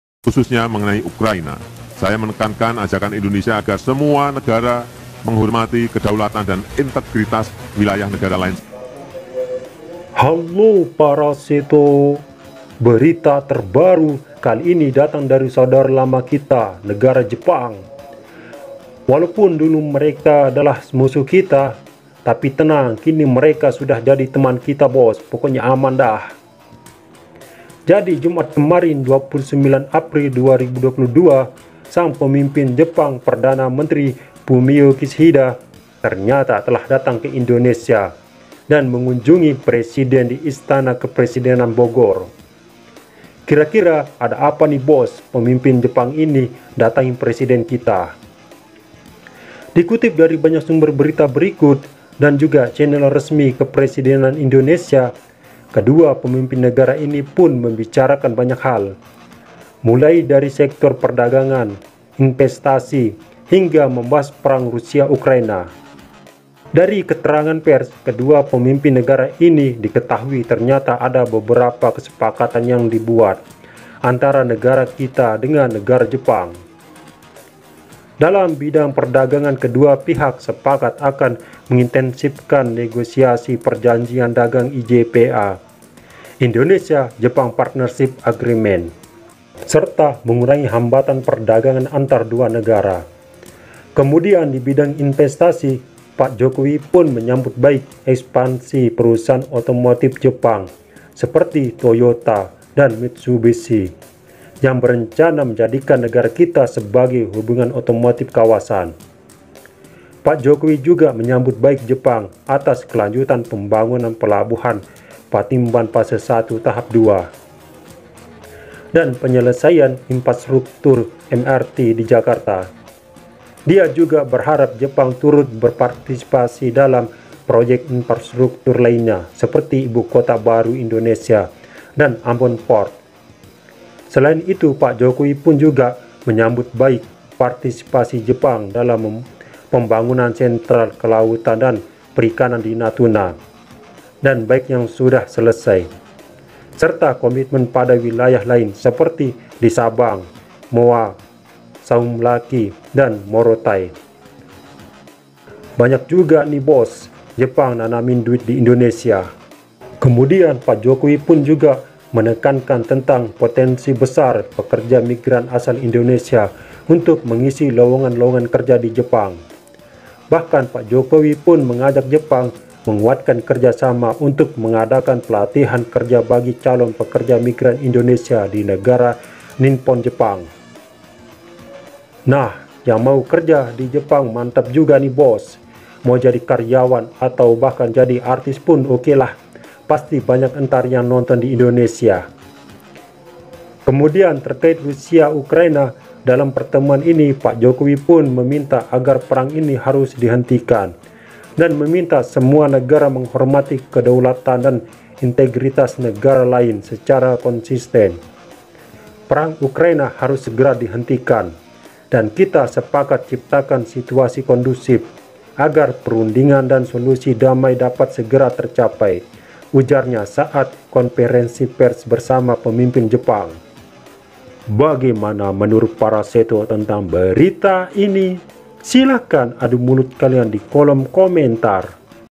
Pemimpin Jepang datang ke Indonesia, sepakati beberapa kerja sama dan menyinggung soal perang
Presiden Joko Widodo menerima secara resmi kunjungan Perdana Menteri Jepang Kishida Fumio, pada Jumat, 29 April 2022, di Istana Kepresidenan Bogor, Jawa Barat. Dalam keterangan pers bersama usai pertemuan bilateral kedua negara, Kepala Negara menyampaikan apresiasinya atas komitmen PM Kishida untuk terus memperkokoh hubungan bilateral Indonesia dan Jepang.

“Saya menyampaikan selamat datang kembali di Indonesia kepada Perdana Menteri Kishida. Suatu kehormatan bagi saya menerima kunjungan Yang Mulia dan saya menyambut baik komitmen Yang Mulia untuk terus memperkokoh hubungan bilateral antara Indonesia dan Jepang,” ucap Presiden.

Presiden Jokowi menyebut bahwa pemerintah Indonesia dan Jepang sepakat untuk terus meningkatkan kerja sama di berbagai bidang. Salah satunya di bidang perdagangan, kedua negara sepakat mengintensifkan negosiasi IJEPA (Indonesia Japan Economic Partnership Agreement) dan mengurangi hambatan perdagangan agar lebih menguntungkan kedua negara.

“Secara khusus, saya menyampaikan permintaan pembukaan akses yang luas bagi produk pertanian dan perikanan Indonesia ke pasar Jepang,” ungkapnya.

Sedangkan di bidang investasi, Presiden Jokowi menyambut baik ekspansi perusahaan otomotif Jepang seperti Toyota dan Mitsubishi yang akan menjadikan Indonesia sebagai hub otomotif untuk kawasan. Meski demikian, Presiden turut berharap adanya tambahan investasi baru Jepang di bidang lainnya serta menjadikan Indonesia sebagai bagian penting dari rantai pasok global industri asal Jepang.

Selain itu, Presiden Jokowi juga menyambut baik kerja sama di bidang infrastruktur dan berharap partisipasi pemerintah Jepang pada proyek-proyek infrastruktur lainnya di Tanah Air, seperti di Ibu Kota Nusantara dan Ambon Port.

“Di sektor kelautan dan perikanan, saya menyambut baik partisipasi Jepang dalam pembangunan sentra kelautan dan perikanan di Natuna dan Biak yang telah selesai dan komitmen pembangunan sentra-sentra serupa di Sabang, Moa, di Saumlaki, dan Morotai,” tutur Presiden.

Sektor lainnya adalah Indonesia dan Jepang turut bekerja sama di sektor energi dan lingkungan hidup. Presiden Jokowi berharap adanya percepatan tindak lanjut kerja sama transisi energi melalui investasi energi baru terbarukan dengan pemerintah Jepang. Presiden juga menekankan pentingnya penguatan kerja sama di sektor ketenagakerjaan, termasuk pelatihan bagi calon pekerja migran Indonesia.

Selain peningkatan kerja sama, Presiden Jokowi dan PM Kishida juga membahas terkait isu kawasan dan isu global, khususnya mengenai Ukraina. Presiden Jokowi kembali menekankan ajakan Indonesia agar semua negara menghormati kedaulatan dan integritas wilayah negara lain secara konsisten.

“Perang Ukraina harus segera dihentikan dan kita bersepakat untuk menciptakan situasi yang kondusif agar perundingan dan solusi damai dapat segera tercapai,” tegas Presiden.

Terkait Myanmar, Presiden Jokowi menekankan pentingnya kerja sama antara ASEAN dan Jepang untuk memastikan bahwa five point of consensus ASEAN dapat dijalankan. Presiden Jokowi dan PM Kishida juga membahas kerja sama Indo Pasifik agar menjadi kawasan yang damai, stabil dan sejahtera.

“Saya menekankan pentingnya membangun strategic trust di kawasan Indonesia dan ASEAN siap melakukan kerja sama dengan para mitra terutama di bidang maritim, konektivitas, dan pencapaian SDGs, serta perdagangan dan investasi,” tandasnya.

Halo para setok jangan lupa tonton video yang lain ok, dan bantu saya untuk mengembangkan Chanel ini dengan cara subscribe channel ini atau share video ini ke akun sosial media kalian.